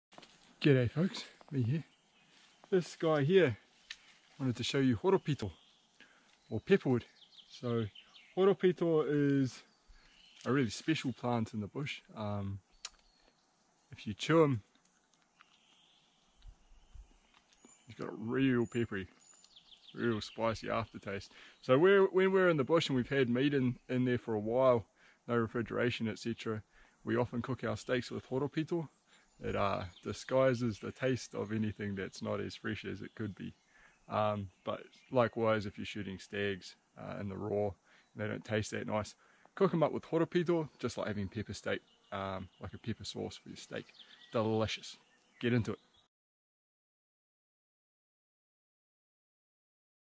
horopito
Hiropito... antibacterial, bad meat disguiser and pepper sauce maker...
Horopito is one of my favourite plants it’s easily identifiable by its leaves. They are often covered by black and red splotches.

Horopito is what I would call a shrub. Growing in darker environments often gullies or under thick canopy. The bark is often covered in black sooty mould.

One of my favourite applications for Horopito is in cooking. I like to use it to make a pepper steak sauce. Also dried it can be crushed up and used as a run for meats. In any case it’s rich in flavour and when you’re in the bush for long periods of time like I am it can help disguise the flavour of meat that is on the turn.

Aside from that Horopito has many of the same qualities as kawakawa. It’s antibacterial and can be used on the skin. Interesting they have the same pepper flavour ae.